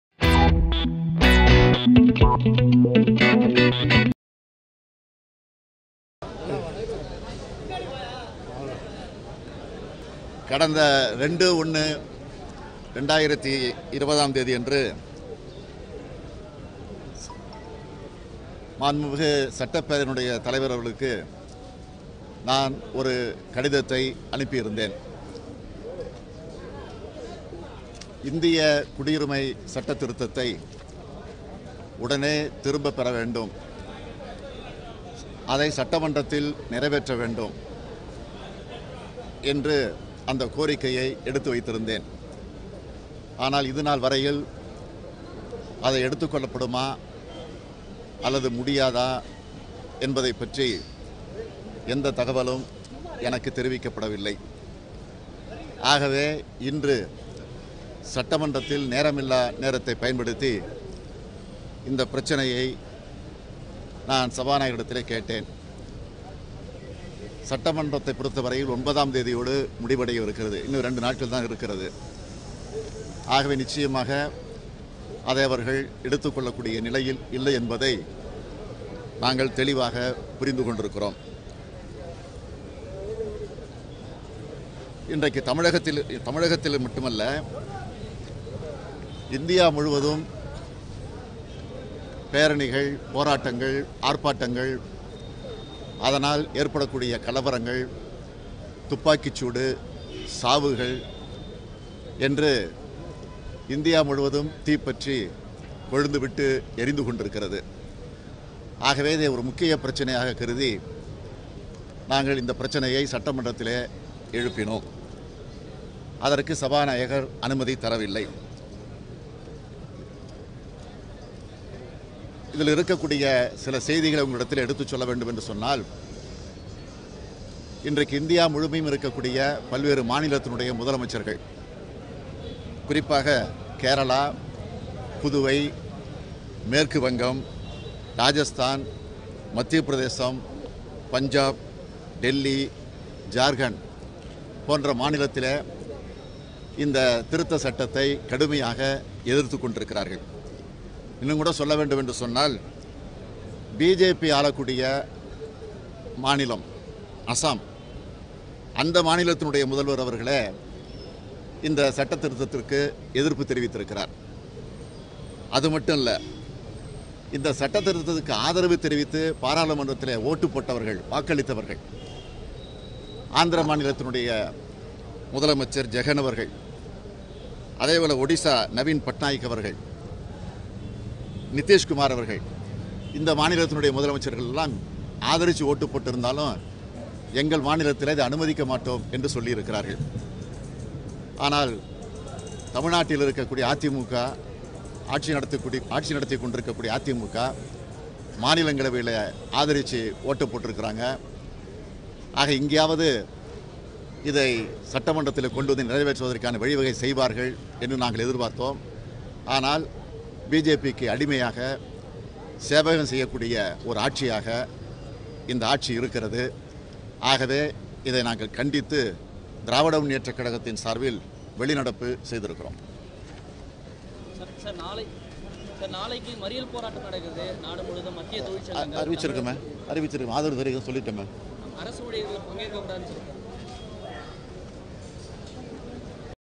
அதிமுக பாஜகவிற்கு அடிமையாக உள்ளது. ||Aiadmk Govt.Working Under Bjp Instructions-M.K.Stalin||DMK
தமிழக சட்டசபையில்  கேள்வி பதில் நேரம் முடிந்த பின்பு எதிர்கட்சி ஒரு தீர்மானத்தை கொண்டு வந்த பேச வாய்ப்பு கோரிகினார். சபாநாயகர் பேச அனுமதி மறுத்த காரணத்தால் தி.மு.க., காஙரகிரஸ், இந்திய முஸ்லீம் லீக், மனித நேய ஜனநாயக கட்சி, முக்குலத்தோர் புலி படை சட்டமன்ற உறுப்பினர்கள் வெளிநடப்பு செய்தனர்.

2.1.2020 அன்று சட்ட பேரவை தலைவருக்கு கடிதம் அனுப்பி இருந்தேன், குடியுரிமை திருத்தச் சட்டத்திற்கு எதிர்ப்பு தெரிவித்து தீர்மானம் கொண்டு வருவது தொடர்பாக.

இது குறித்து இன்று நாங்கள் பேரவையில் கேட்டோம்,ஆனால் சபாநாயகர் ஏற்கவில்லை.

தமிழகம் முழுவதும் மட்டும் அல்ல இந்தியா முழுவதும் பேரணி,போராட்டம்,
பிரச்சினைகளகள் ஏன் சாவுகள் கூட விழுந்து கொண்டு உள்ளது,நாடு முழுவதும் இந்த விவகாரம் தீப்பற்றி எரிகிறது.

எனவே இதனை உடனடியாக கவனத்தில் கொள்ள வேண்டும் என்று கேட்டுக்கொண்டேன்.

பி.ஜே பி ஆளும் அசாமில் கூட எதிர்ப்பு தான்,அதே போல் ஆந்திர முதல்வர்,ஒடிசா முதல்வர்கள் எங்கள் மாநிலத்தில் அனுமதிக்க மாட்டோம் என கூறி உள்ளனர்,ஆனால் அதிமுக ஆதரவு தெரிவித்து உள்ளது,அதிமுக பாஜகவிற்கு அடிமையாக உள்ளது.

எனவே இதற்கு எதிர்ப்பு தெரிவித்து நாங்கள் வெளி நடப்பு செய்துள்ளோம்.